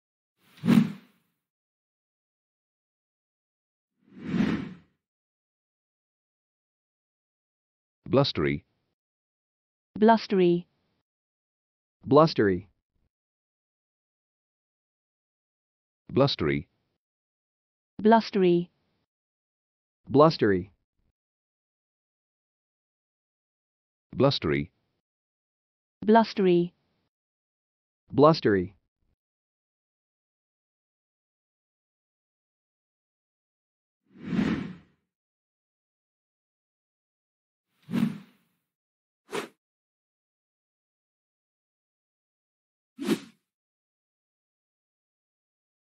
How to pronounce "Blustery"?
Learn how to pronounce "Blustery" in English, improve your accent and sound like a native speaker.

Blustery is an adjective meaning "with strong winds":
"a blustery day"
"blustery weather"
Related words and phrases:
Wind airstream anemometer blast blowy breeze stormy windy